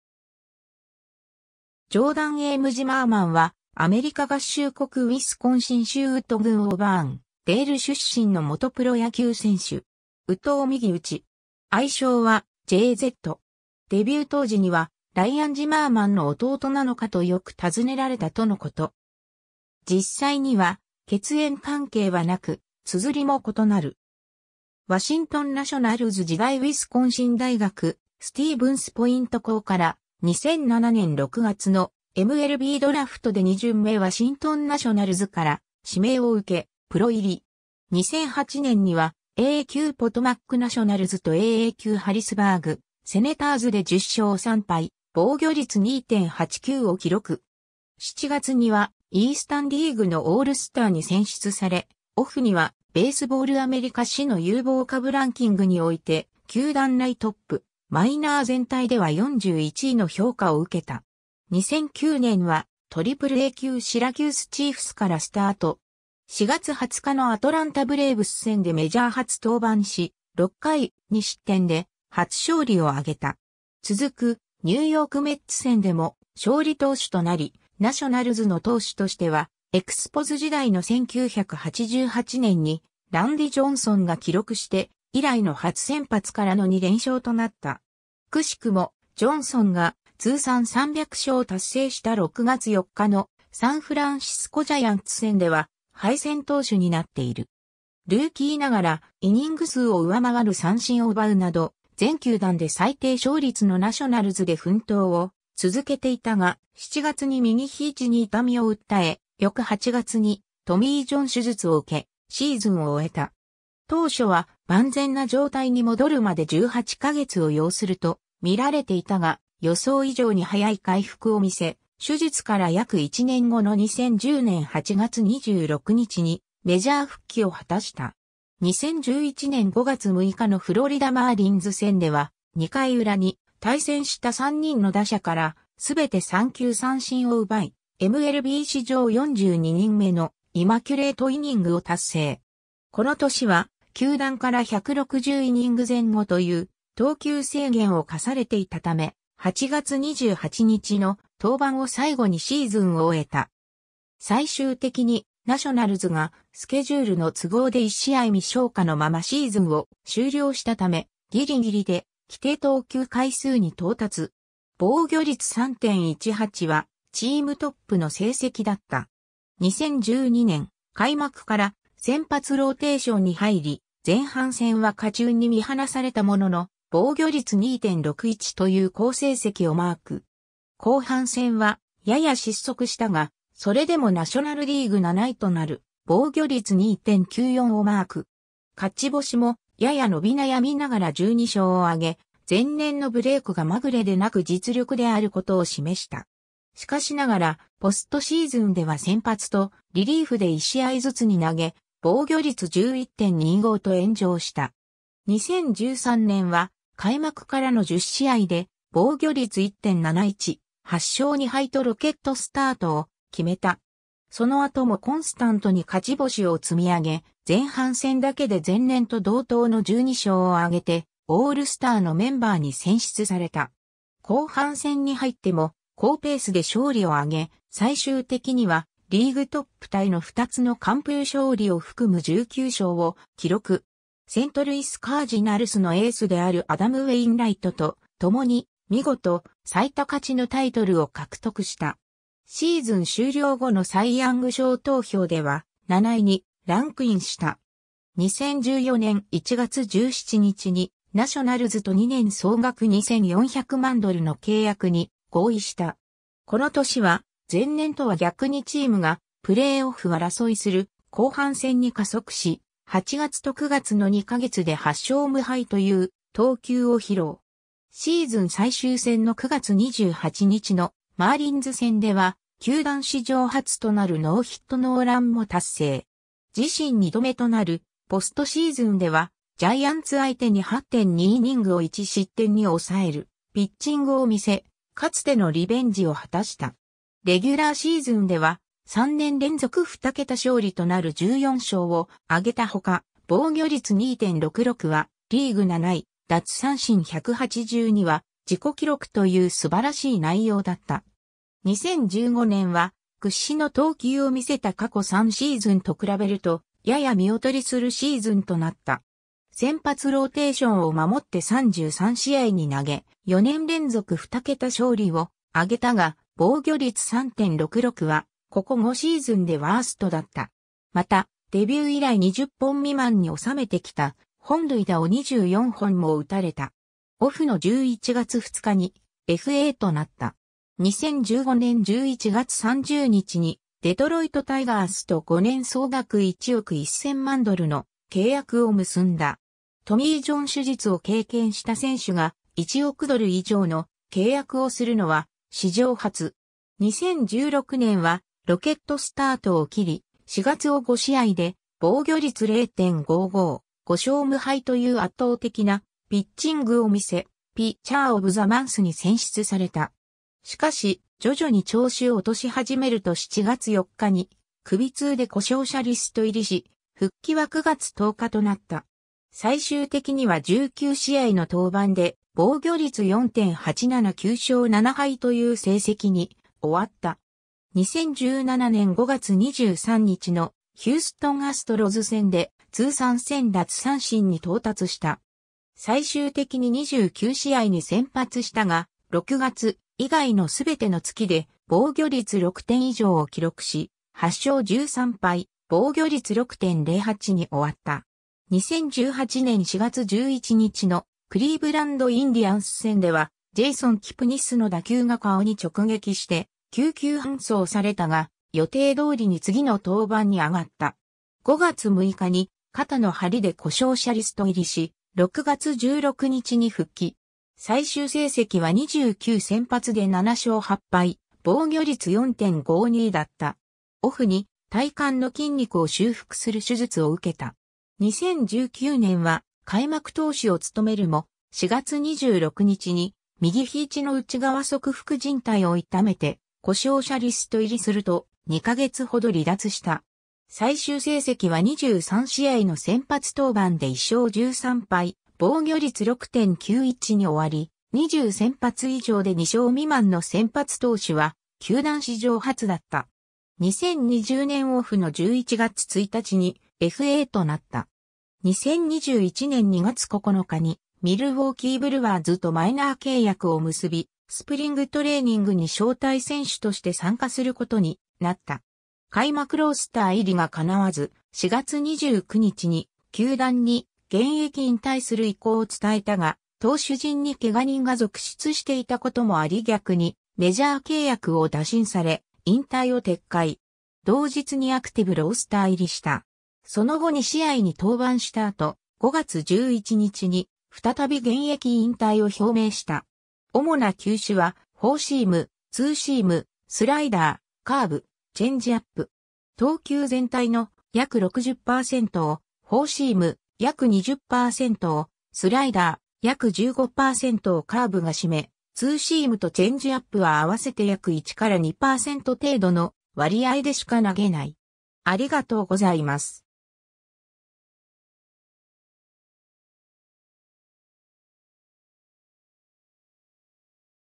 ジョーダン・ジマーマン
ジョーダン・M・ジマーマン（Jordan M. Zimmermann, 1986年5月23日 - ）は、アメリカ合衆国ウィスコンシン州ウッド郡オーバーンデール出身の元プロ野球選手（投手）。
右投右打。
愛称はJZ。
 デビュー当時には、「（チームメイトであった）ライアン・ジマーマン（Ryan Zimmerman）の弟なのかとよく尋ねられた」とのこと。
実際には血縁関係はなく、綴りも異なる。
 2007年6月のMLBドラフト2巡目(全体67位)でワシントン・ナショナルズから指名を受け、プロ入り。
 2008年にはA+級ポトマック・ナショナルズとAA級ハリスバーグ・セネターズで10勝3敗・防御率2.89を記録。
7月にはイースタンリーグのオールスターに選出され、オフには「ベースボール・アメリカ」誌の有望株ランキングにおいて、球団内トップ、マイナー全体では41位の評価を受けた。
 2009年はAAA級シラキュース・チーフスからスタート。
4月20日のアトランタ・ブレーブス戦でメジャー初登板し、6回2失点で初勝利を挙げた。
続くニューヨーク・メッツ戦でも勝利投手となり、ナショナルズの投手としてはエクスポズ時代の1988年にランディ・ジョンソンが記録して以来の初先発からの2連勝となった。
奇しくも、ジョンソンが通算300勝を達成した6月4日のサンフランシスコ・ジャイアンツ戦では敗戦投手になっている。
ルーキーながらイニング数を上回る三振を奪うなど、全球団で最低勝率のナショナルズで奮闘を続けていたが、7月に右肘に痛みを訴え、翌8月にトミー・ジョン手術を受け、シーズンを終えた。
 当初は万全な状態に戻るまで18カ月を要すると見られていたが、予想以上に早い回復を見せ、手術から約1年後の2010年8月26日にメジャー復帰を果たした。
 2011年5月6日のフロリダ・マーリンズ戦では、2回裏に対戦した3人の打者からMLB史上42人目の三者連続三球三振を達成。
この年は球団から160イニング前後という投球制限を課されていたため、8月28日の登板を最後にシーズンを終えた。
最終的に、ナショナルズがスケジュールの都合で1試合未消化のままシーズンを終了したため、ギリギリで規定投球回数に到達。
防御率3.18はチームトップの成績だった。
 2012年は開幕から先発ローテーションに入り、前半戦は勝ち運に見放されたものの、防御率2.61という好成績をマーク。
後半戦は、やや失速したが、それでもナショナルリーグ7位となる防御率2.94をマーク。
勝ち星も、やや伸び悩みながら12勝を挙げ、前年のブレイクがまぐれでなく実力である事を示した。
しかしながら、ポストシーズンでは先発とリリーフで1試合ずつに投げ、防御率11.25と炎上した。
 2013年は開幕からの10試合で防御率1.71、8勝2敗とロケットスタートを決めた。
その後もコンスタントに勝ち星を積み上げ、前半戦だけで前年と同等の12勝を挙げてオールスターゲームに選出された。
後半戦に入っても好ペースで勝利を挙げ、最終的にはリーグトップタイの2つの完封勝利を含む19勝を記録。
セントルイス・カージナルスのエースであるアダム・ウェインライトと共に見事、最多勝のタイトルを獲得した。
シーズン終了後のサイ・ヤング賞投票では、7位にランクインした。
 2014年はシーズン開幕前の1月17日にナショナルズと2年総額2400万ドルの契約に合意した。
この年は、前年とは逆にチームがプレーオフ争いする後半戦に加速し、8月と9月の2ヵ月で8勝無敗という投球を披露。
シーズン最終戦の9月28日のマーリンズ戦では球団史上初となるノーヒットノーランも達成。
自身2度目となるポストシーズンでは、ジャイアンツ相手に8.2イニングを1失点に抑えるピッチングを見せ、かつてのリベンジを果たした。
レギュラーシーズンでは、3年連続二桁勝利となる14勝を挙げたほか、防御率2.66はリーグ7位、奪三振182は自己記録という素晴らしい内容だった。
 2015年は、屈指の投球を見せた過去3シーズンと比べると、やや見劣りするシーズンとなった。
先発ローテーションを守って33試合に投げ、4...